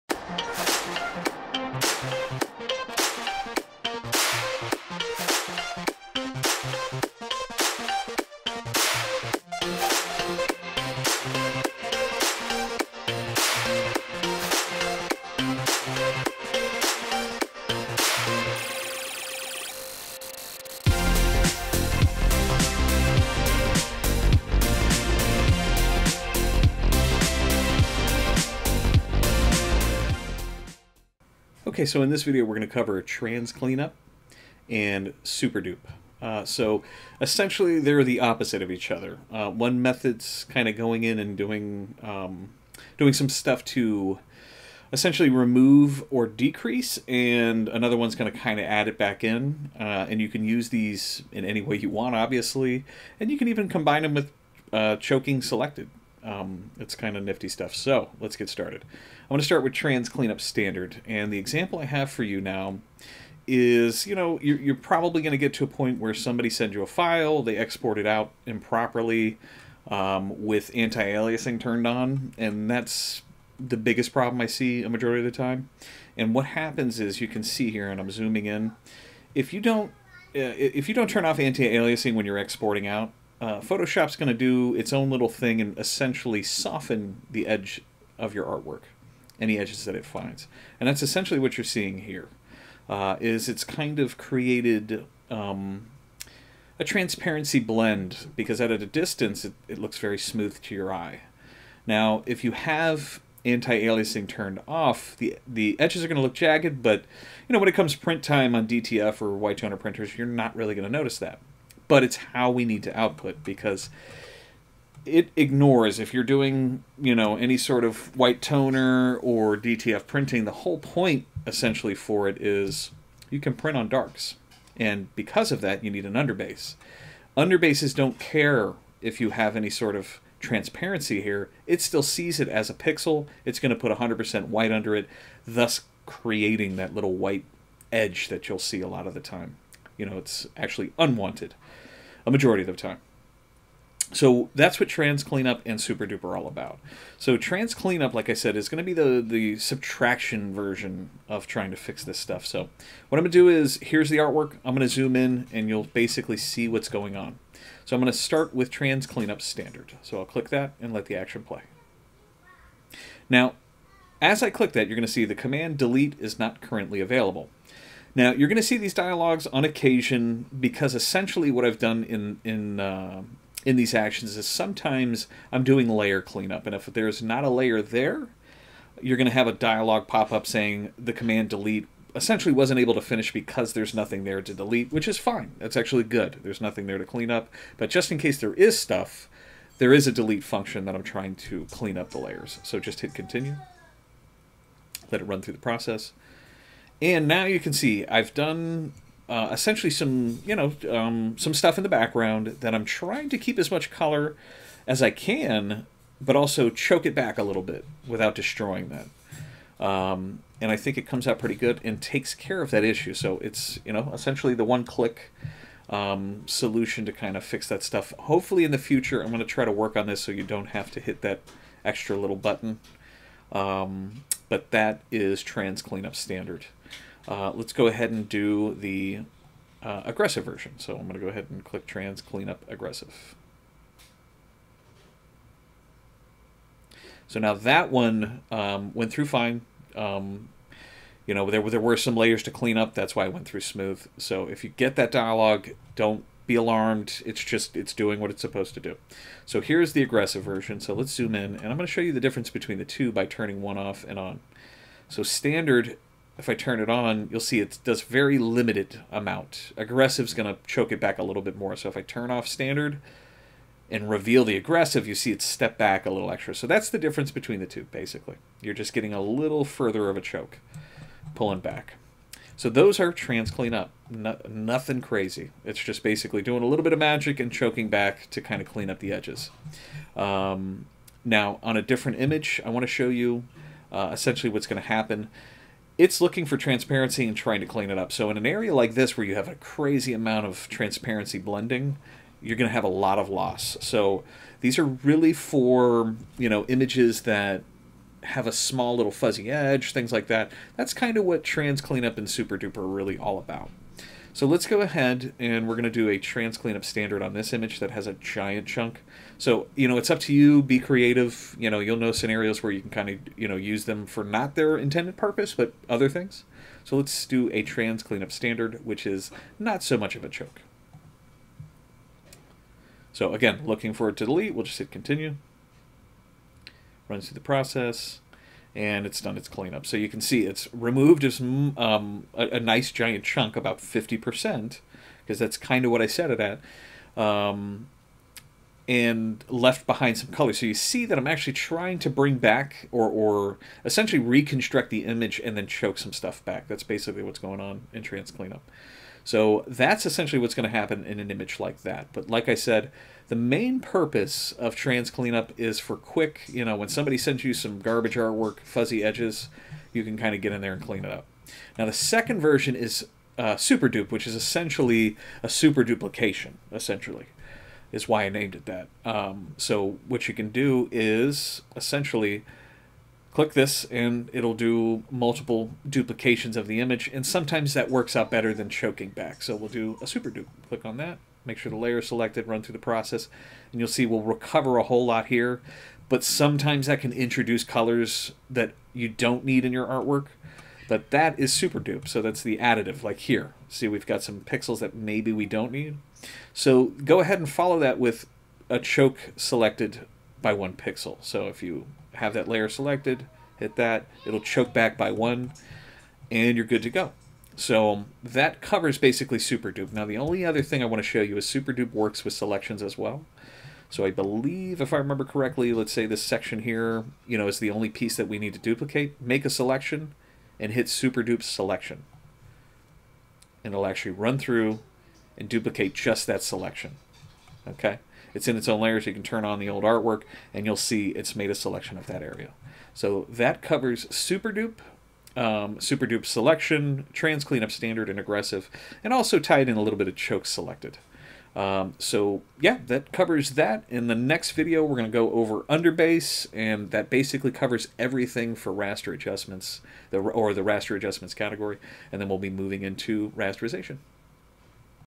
DTF Rasterization Kit Tutorial - 7 Transparency Cleanup
Here I show you how to use the Trans Clean-up action options in the Rasterization Kit by Pixel Moshpit.